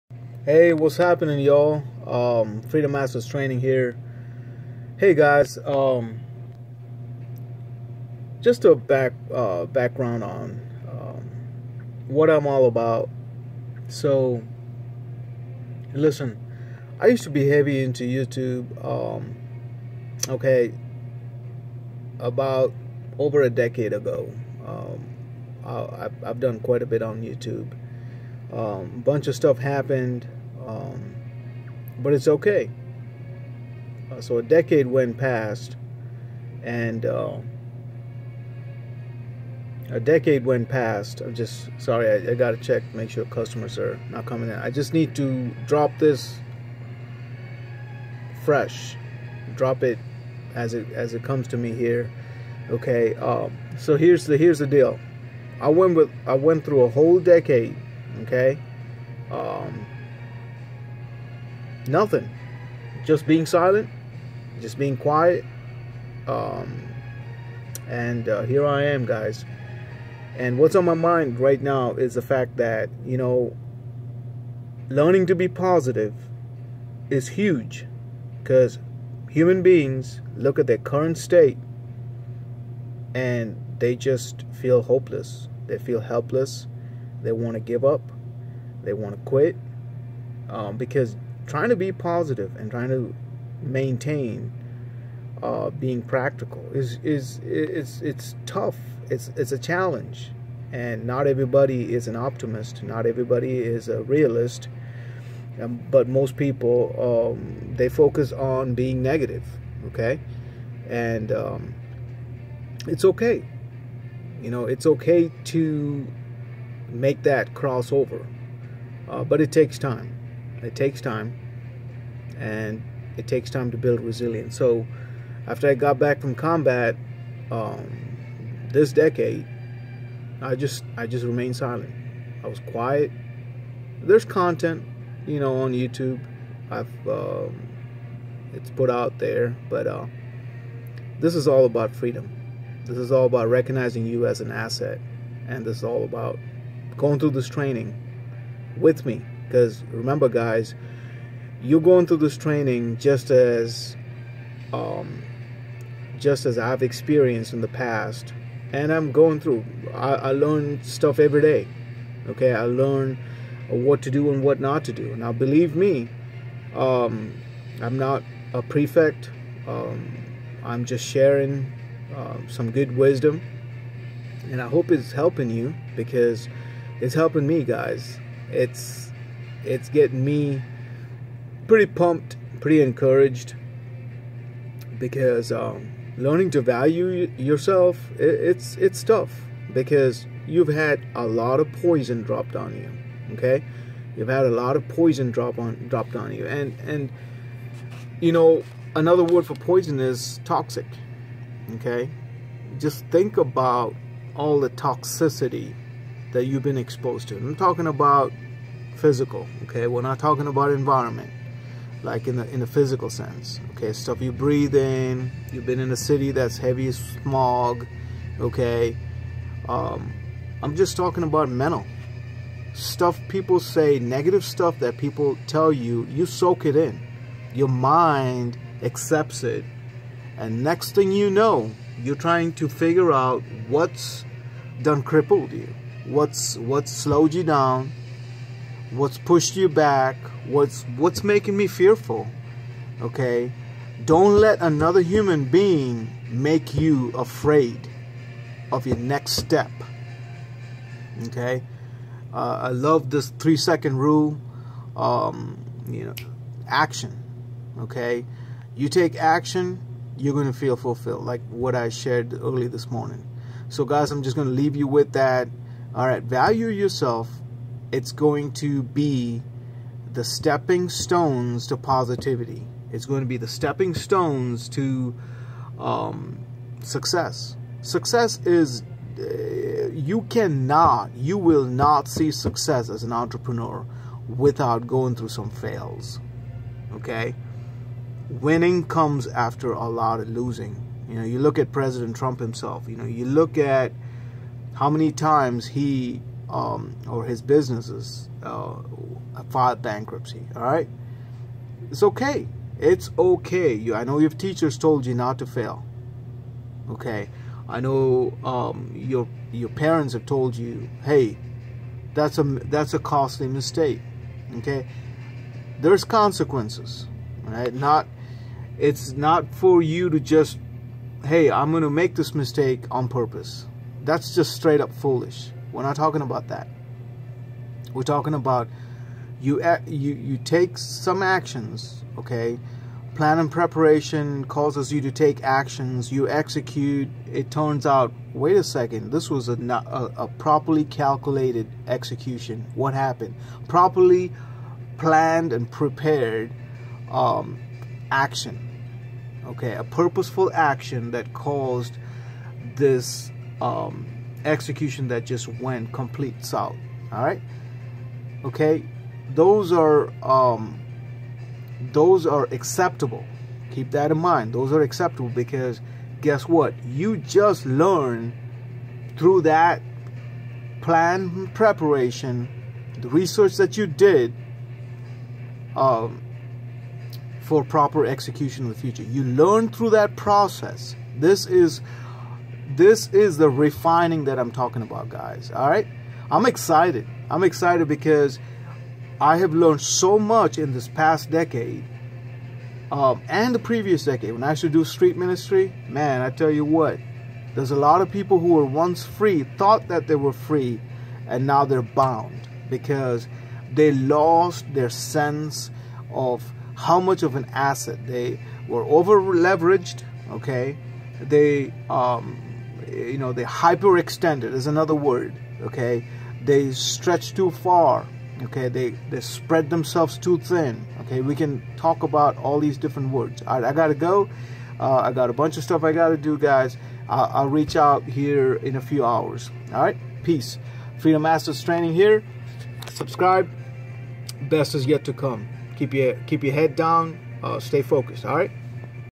Be Positive be Practical for this is Mental Resilience Value Yourself today. Freedom FAT - Vlog 13
Work on your mind and teach it to be positive. Persevere and stop focusing on problems. People in the past may have been poisonous to your mind but it is time to get your mind back on track. Focus on solutions. Keep your head down and get written down. The best is yet to come. Make it great :-)